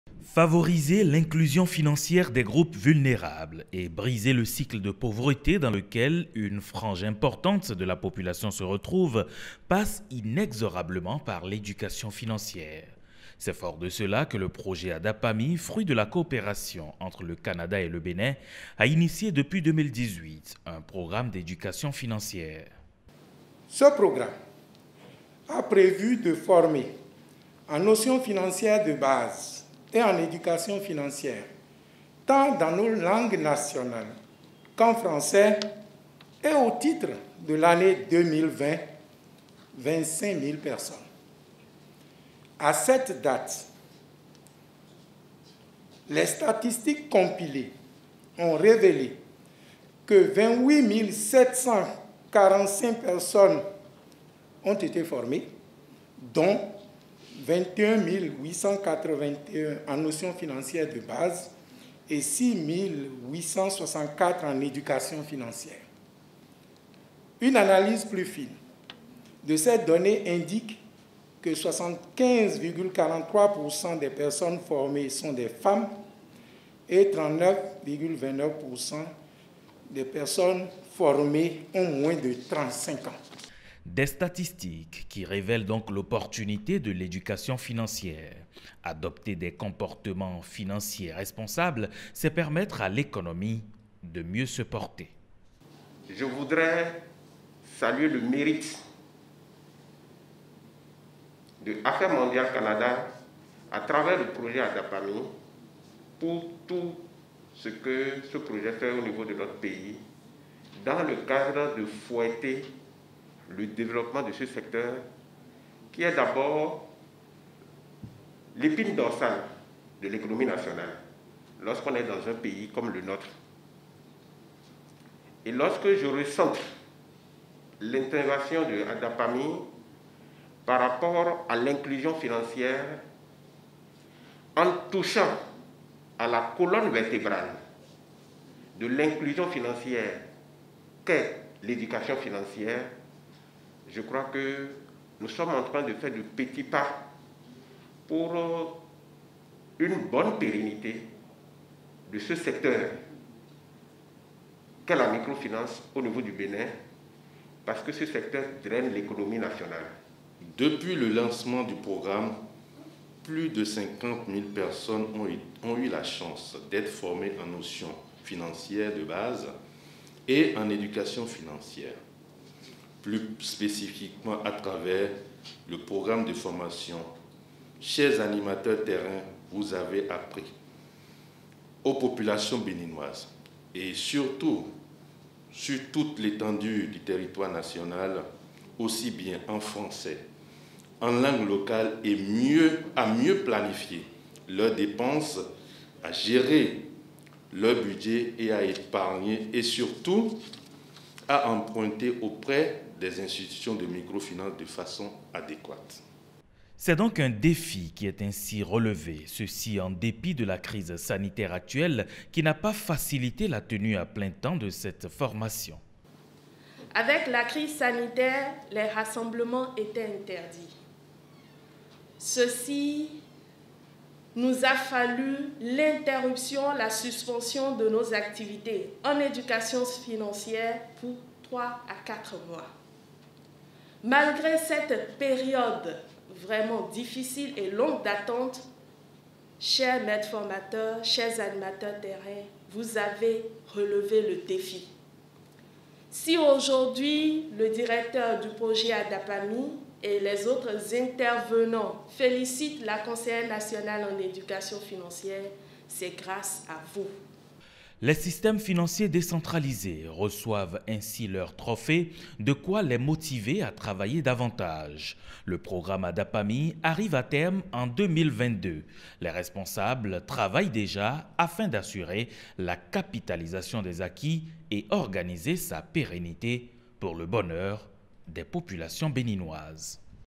Education financière : reconnaissance des meilleures performances 2020 des SFD
L’éducation financière constitue un pilier essentiel dans le processus d’inclusion financière. Au Bénin, le programme Adapami mis en œuvre depuis 2018, forme les responsables des systèmes financiers décentralisés. Pour le compte de l’année 2020, plusieurs SFD ont été formés et leurs performances ont été reconnues, à travers des trophées qui leur ont été remis. Les précisions de Ricardo Loïc Kpékou et Alexis Sègbèmon